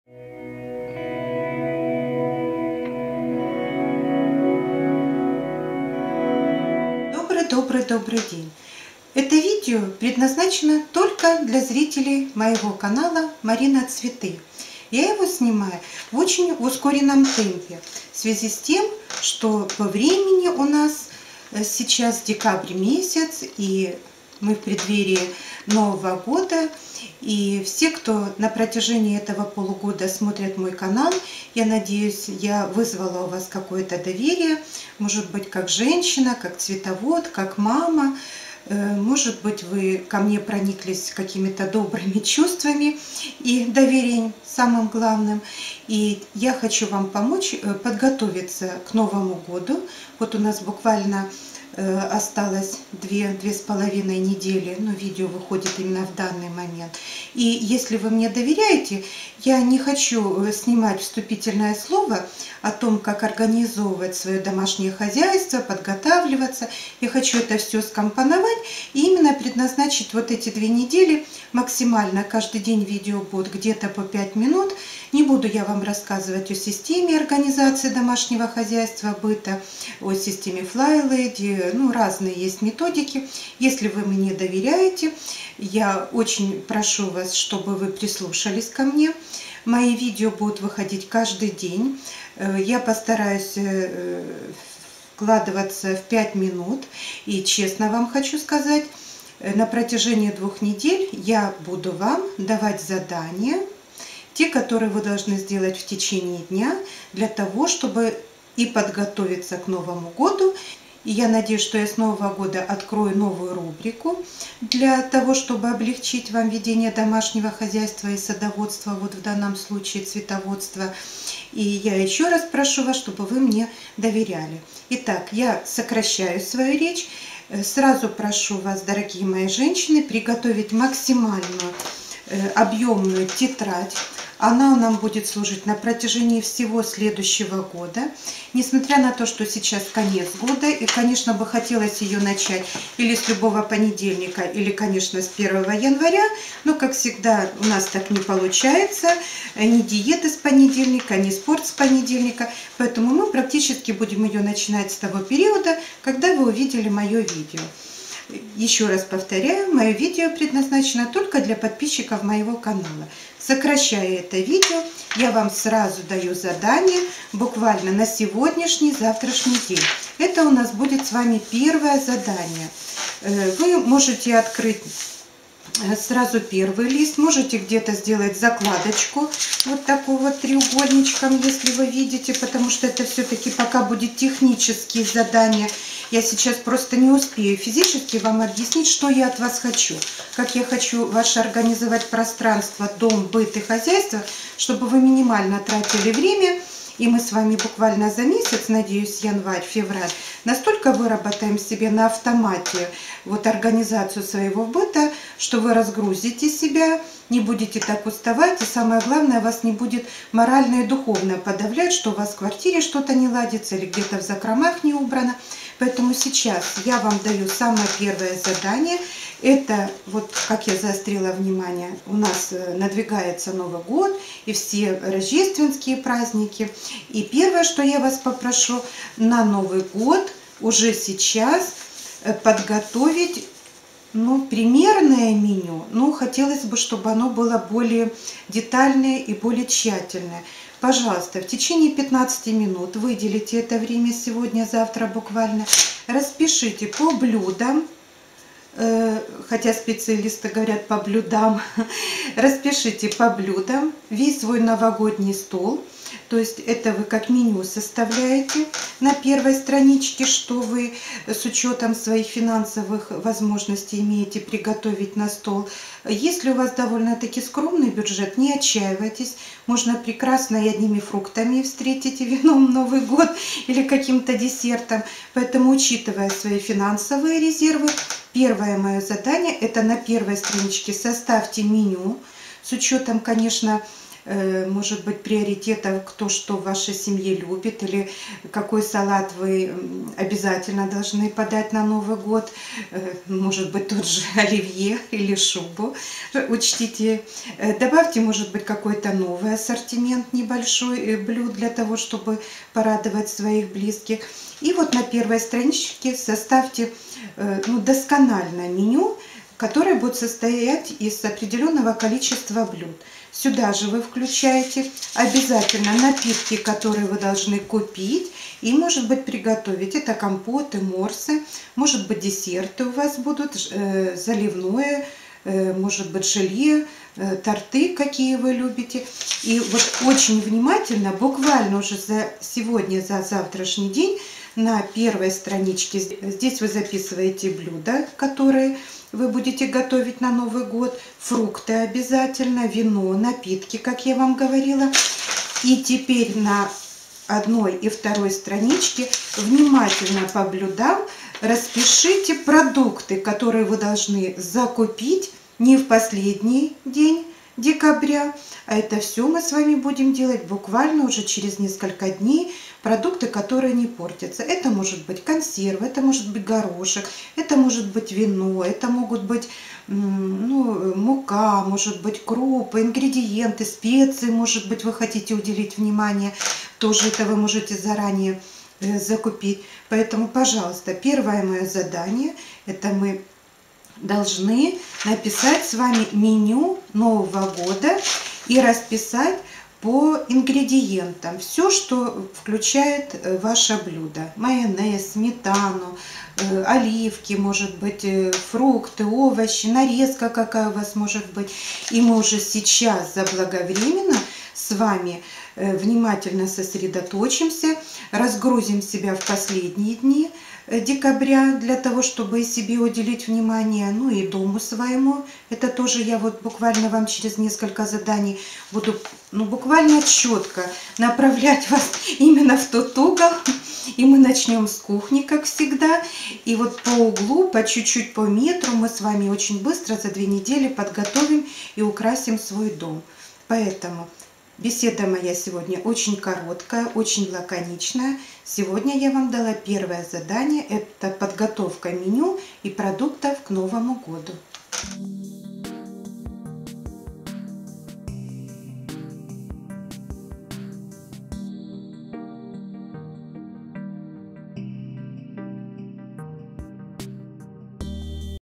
Подготовка к Новому Году! Составление меню и списка продуктов к закупке. Задание №1
Совсем скоро Новый Год! И я предлагаю подготовится к нему заранее. Благодаря правильной организации, подготовка к празднику будет легкой, незаметной и приносящей удовольствие. Вы не будете себя чувствовать уставшей, замотанной, при этом у Вас все будет готово заранее и Вы сможете с удовольствием и легкостью встретить Новый Год. Каждый день я буду давать Вам небольшие задания, которые помогут Вам с легкостью и заранее подготовится к Новому Году. В Новогоднюю ночь Вы не будете, как раньше, уставшей и замученной, а будете сиять и радоваться этому празднику!
Подготовка к Новому году! Составление меню и списка продуктов к закупке. Задание №1. При организации подготовки к празднику будет использоваться незаменимая система "Флайледи".